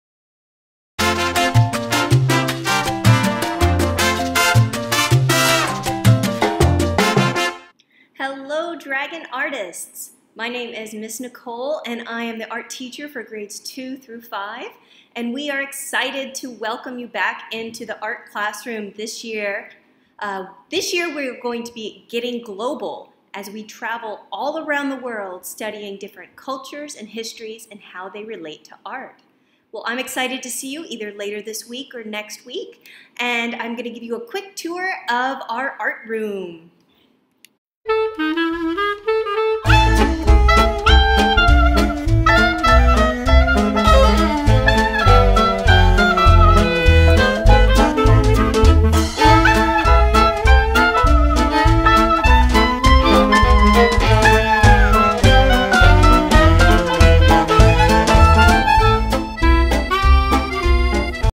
Dragon Artists: Get Global 2020-21
Introduction for Dragon Artists to the 2020-21 school year.